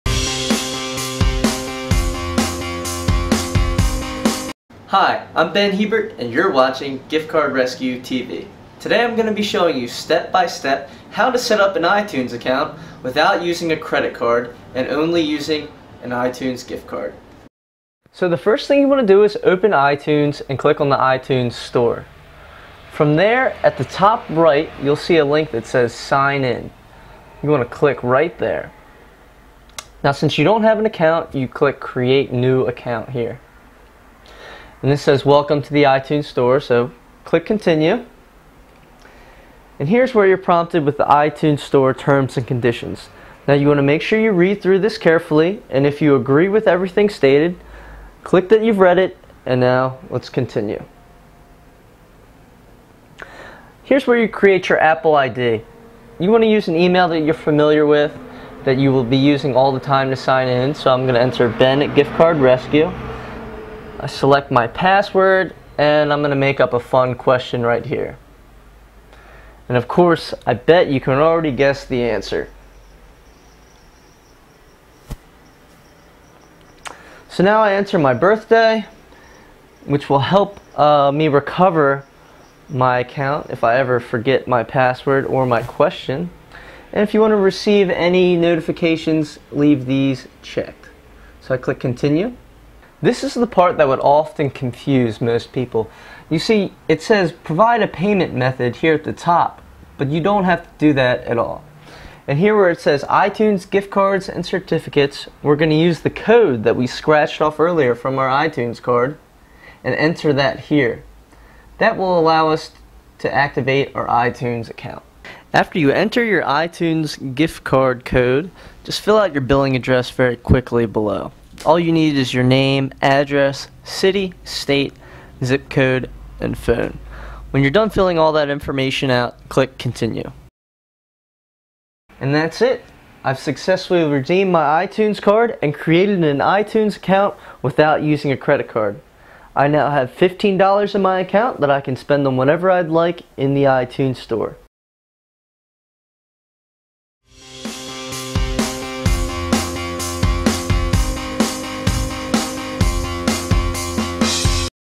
Set up iTunes Account with Gift Card - Without Credit Card
How to set up an iTunes Account without a credit card using an iTunes gift card